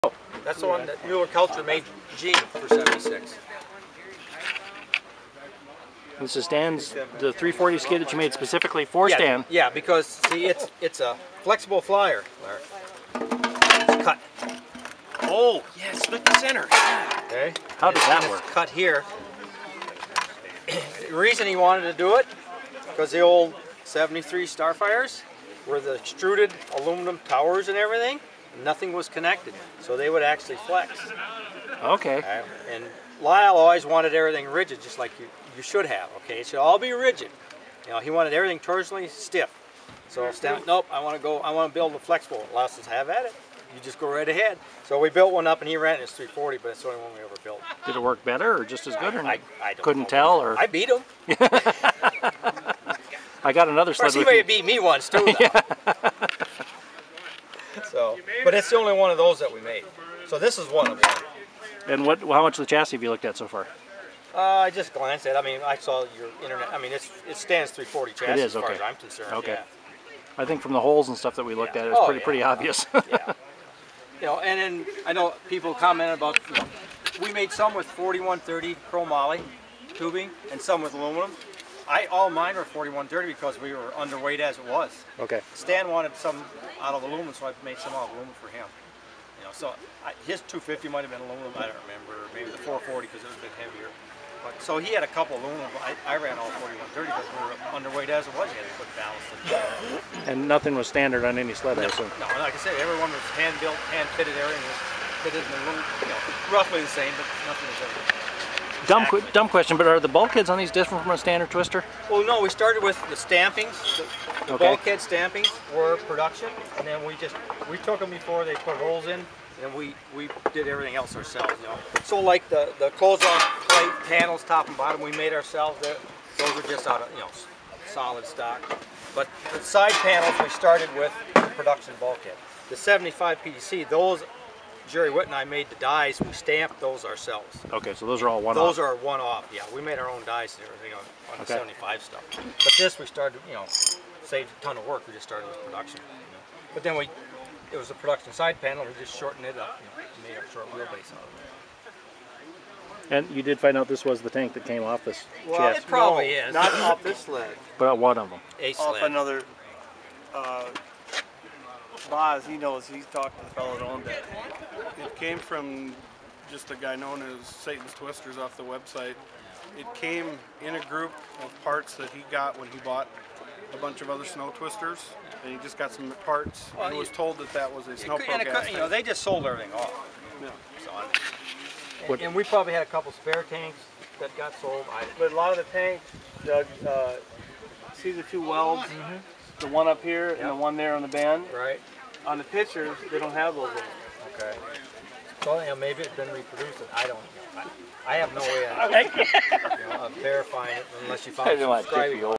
Doug Hayes on the 1976 Merc SnoPro's Part I
Doug Hayes reviews his brother Stan's 1976 340 oval racing Mercury SnoPro snowmobile that was recently found in a barn in Iowa. Mercury was involved with professional oval snowmobile racing in 1975 and 1976 with both Stan and Doug driving for the manufacturer.